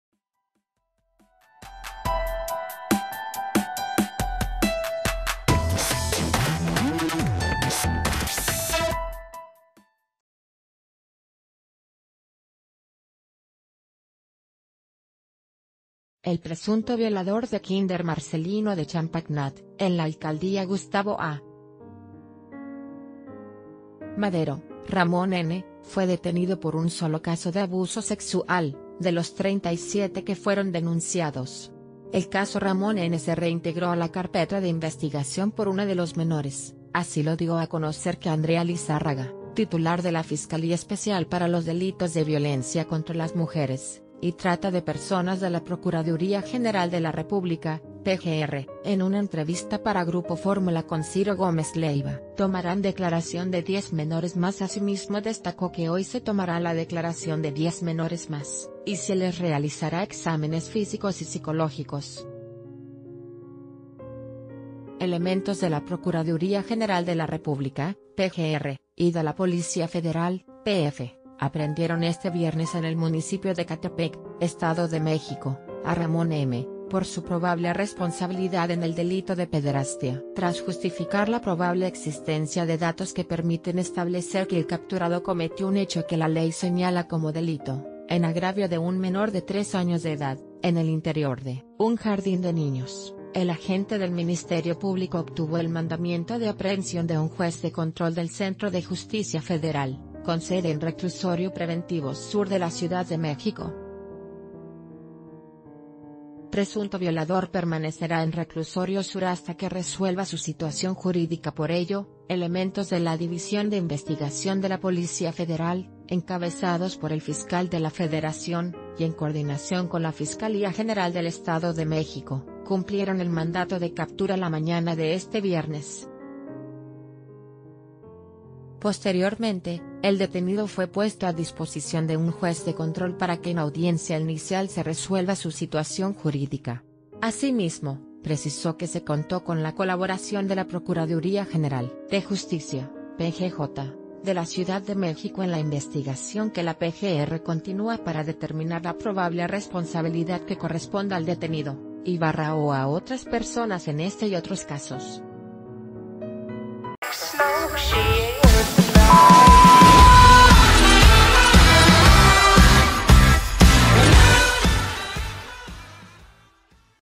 Presunto violador de kinder en GAM fue detenido por un sólo caso de abuso sexual
El presunto violador de kinder Marcelino de Champagnat, en la alcaldía Gustavo A. Madero, Ramón “N”, fue detenido por un solo caso de abuso sexual, de los 37 que fueron denunciados.“El caso Ramón “N” se reintegró a la carpeta de investigación por uno de los menores”, así lo dio a conocer que Andrea ...